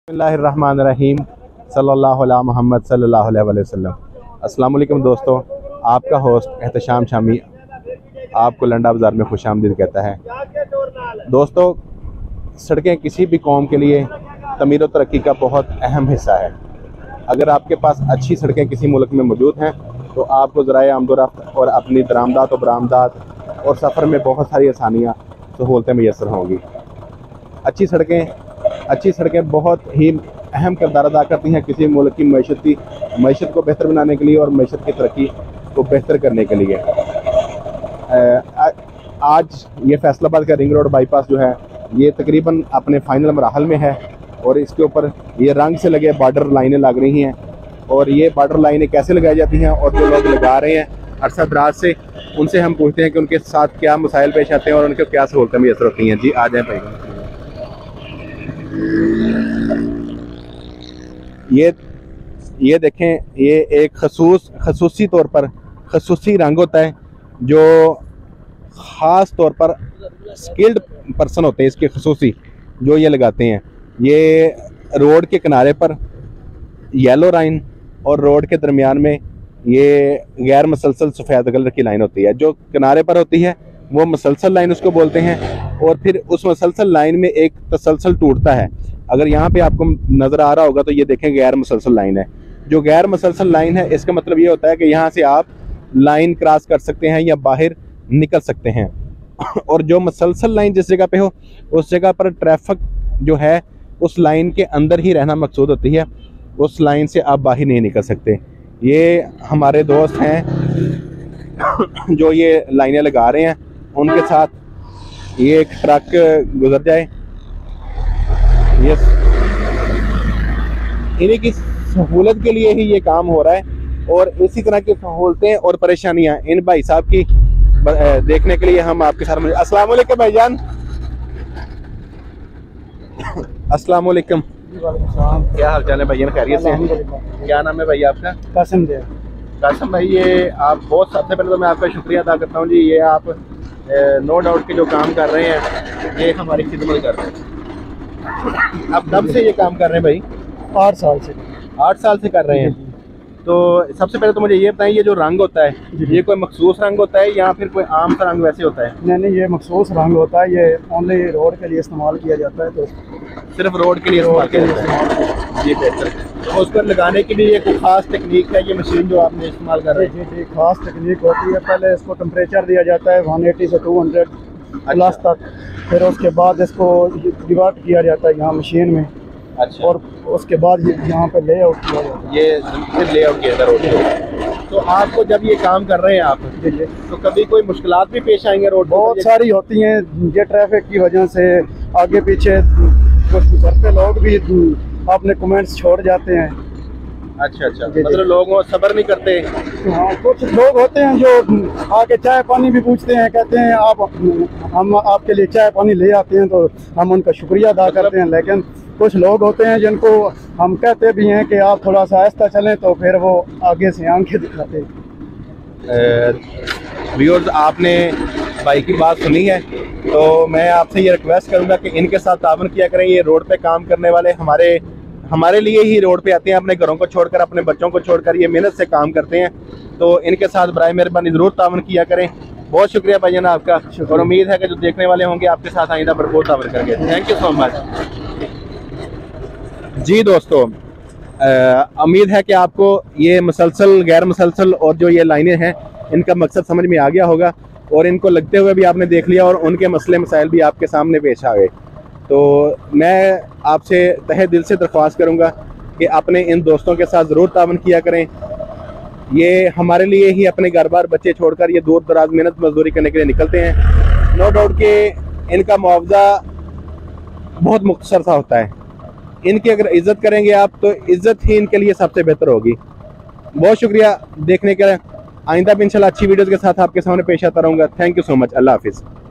Problems Of Road Workers | انکے مسائل سنیں اور انکو حل کریں اپکی مہربانی ہوگی | Landa Bazar
دوستو اس ویڈیو میں اپ کی میں ملاقات کرواؤں گا روڈ پر کام کرنے والے محنت کشوں سے جب اپ انکے پاس سے گزرتے ہیں تو انکو کیا کیا مسائل دیکھنا پرتے ہیں مہربانی کر کے انکے مسائل سنیں اور انکو حل کریں
جزاک اللّه